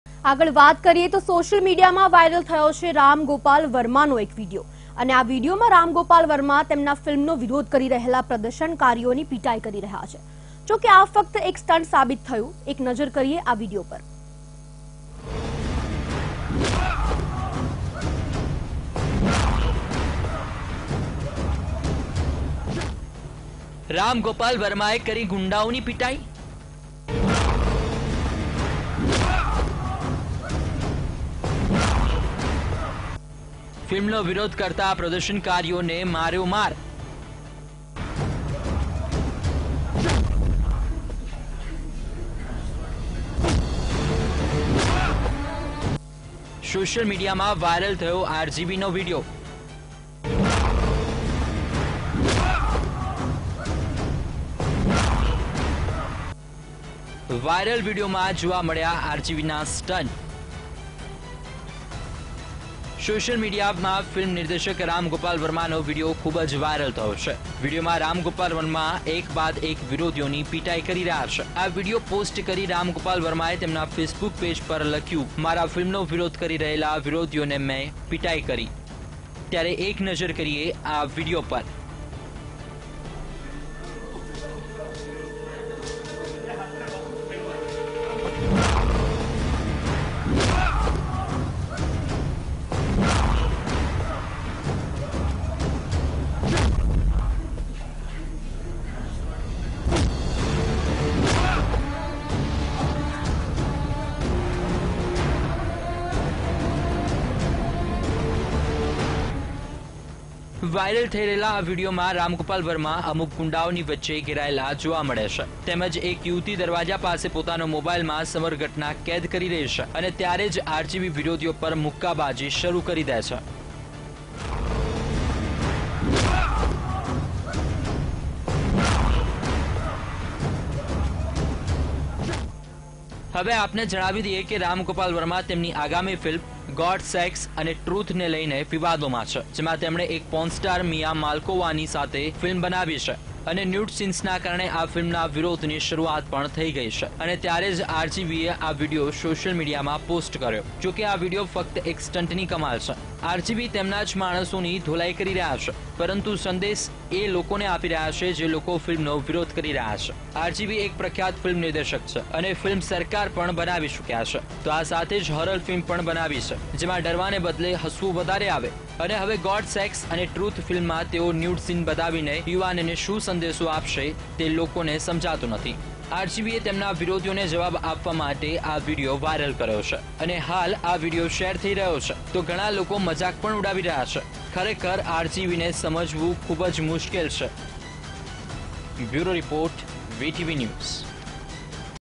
Viral Truth | Ram Gopal Varma Fighting with people protesting his Film? | Vtv News
Viral Truth | Ram Gopal Varma Fighting with people protesting his Film?